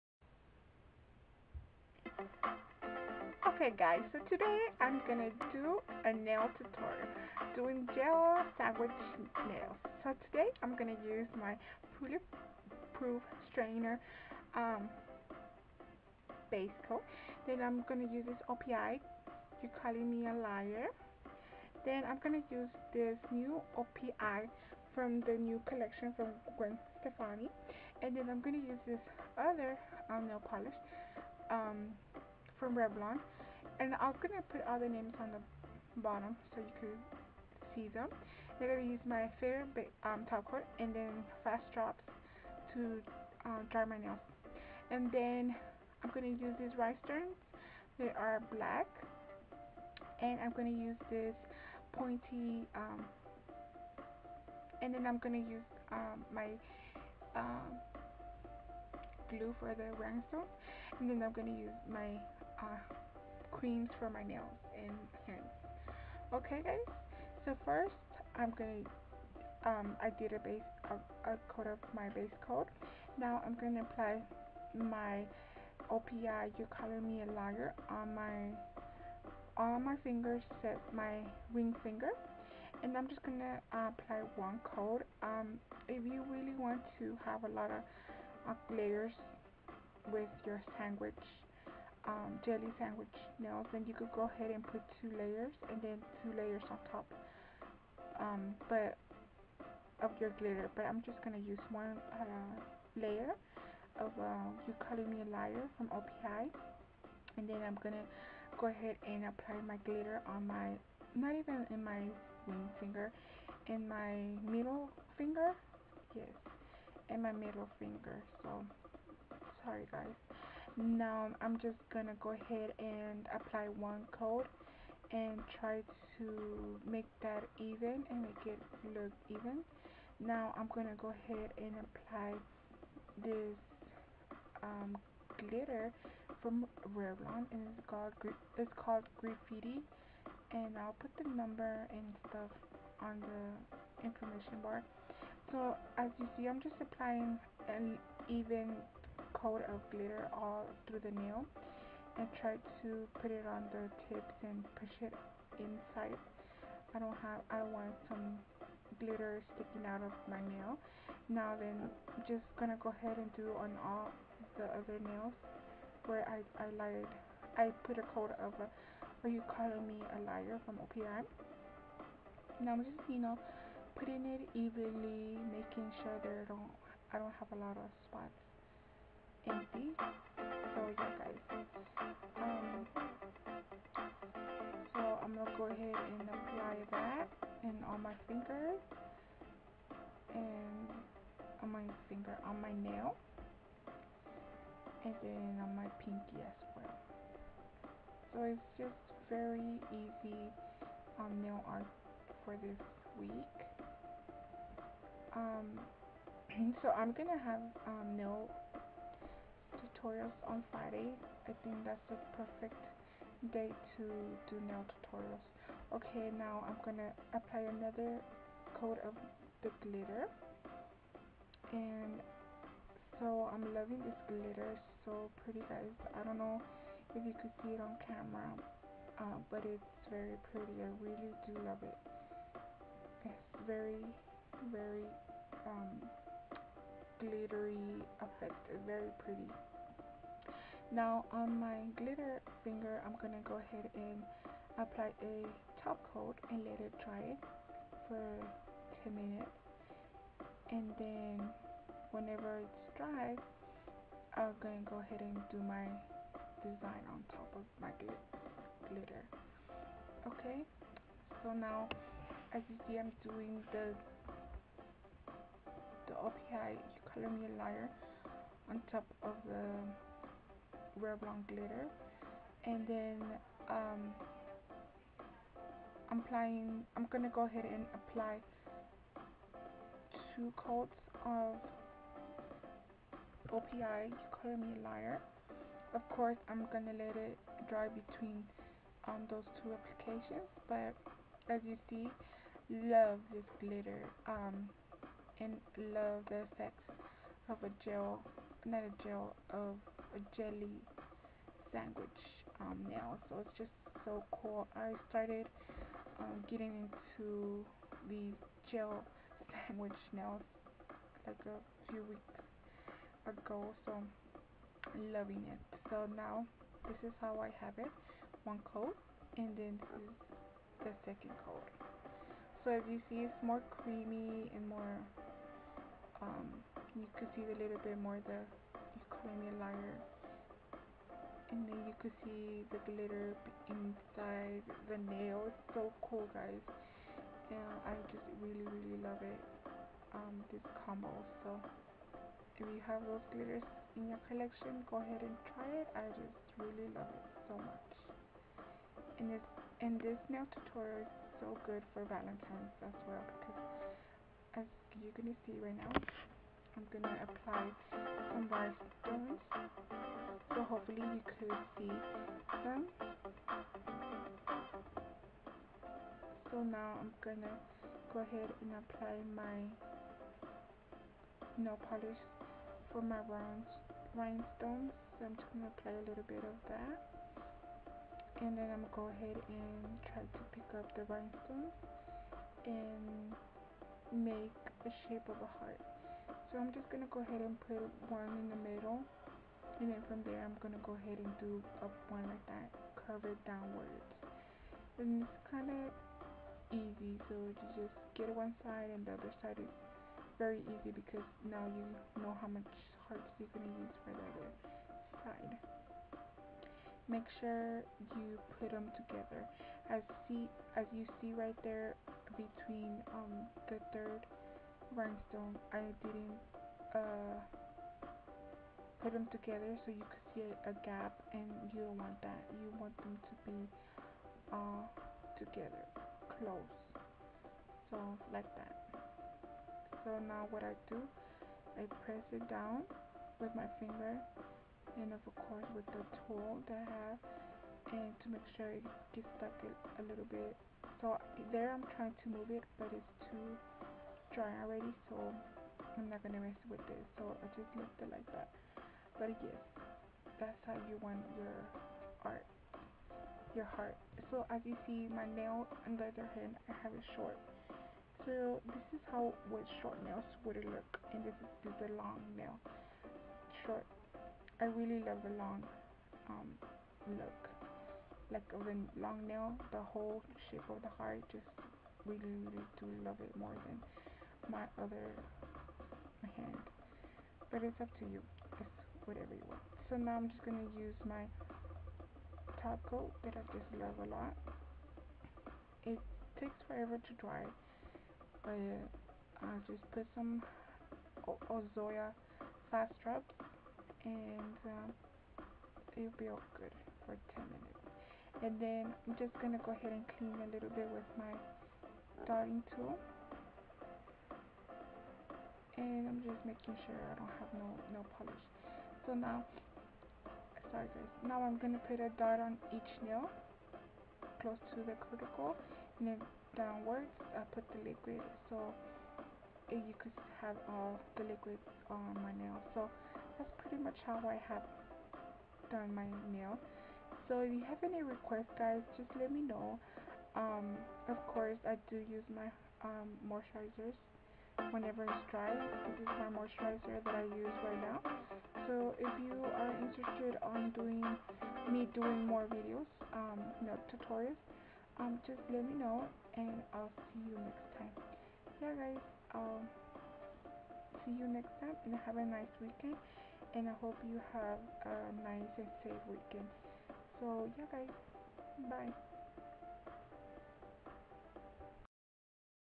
Jelly Sandwich Nail Tutorial First One
What I Use :
My base coat Nutra Nail Bullet-proof Strength

For Jelly Sandwich Nails I Use

OPI You Callin' Me A Lyre?
Revlon 678 Graffiti Top Coat

OPI In True Stefani Fashion

Nail Art I Use

For Rhinestones Only!! Polish

3mm Black Rhinestones

Nail Care

Zoya Fast Drops
Hard as Hoof Nail Strengthening Cream
F.H.F Hello Yellow Shea Butter Hand Cream

              o
              U

I bought all this products with all me money :)